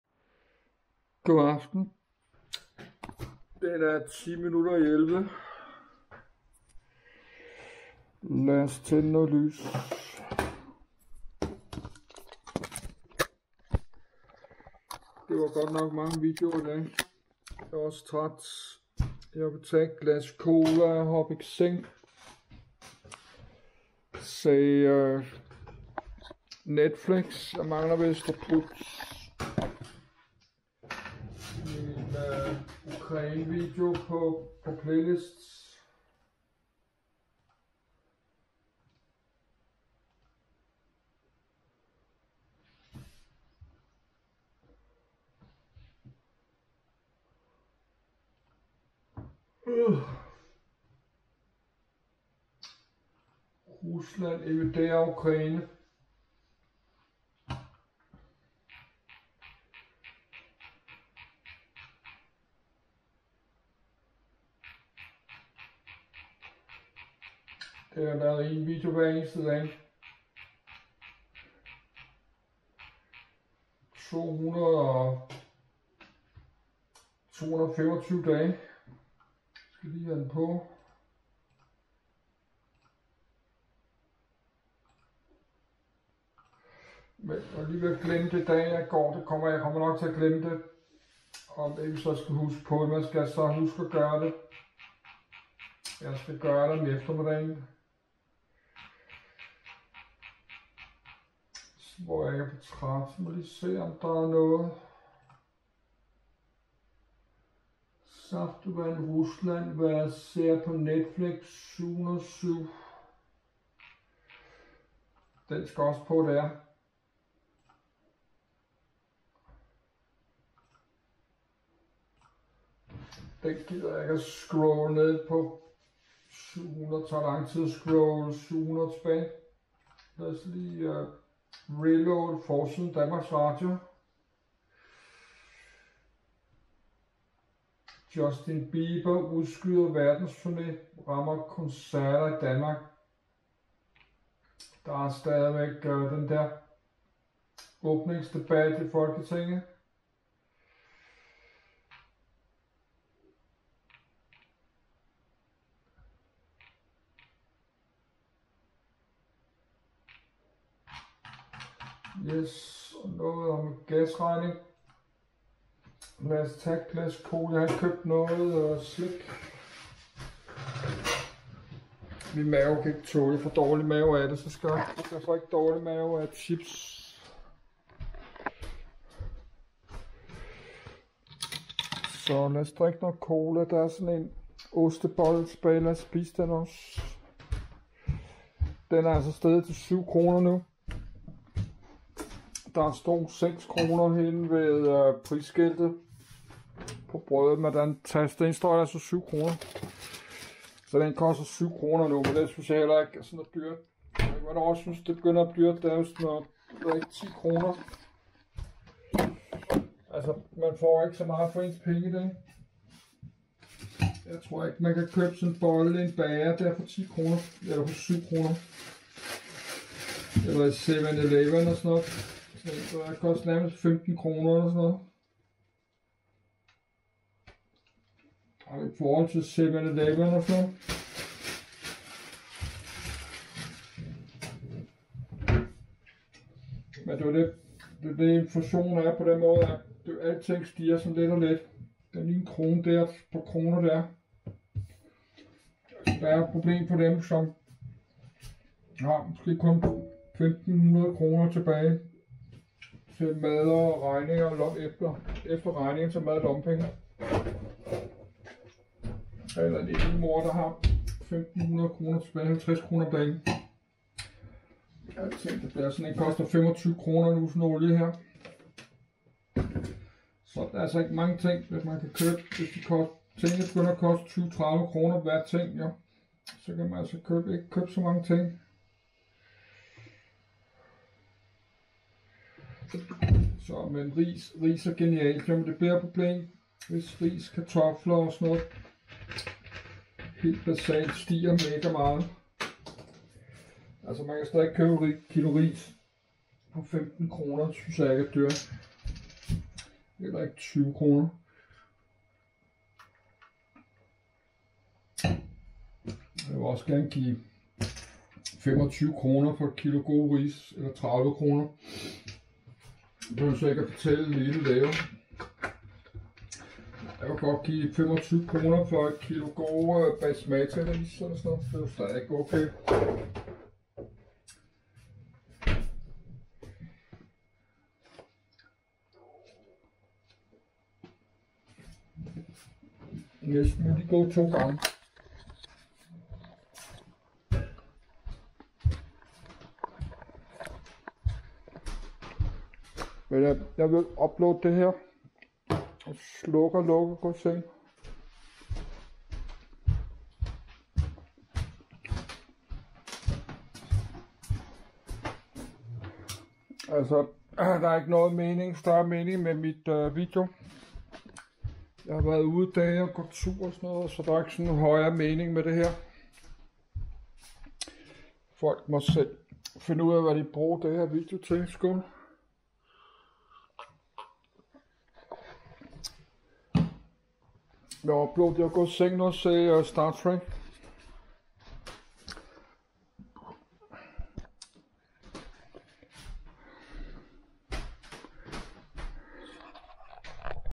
MongoTV_9852 - Godnat - Kom Godt Igennem TORSDAG NAT
Godnat :-) kom godt igennem TORSDAG NAT. Klokken den er 23. Jeg vil gå i seng, og se den gamle Star Trek fra år 1993 på Netflix. Det var mange video, som jeg fik uploadet i dag. zzzzz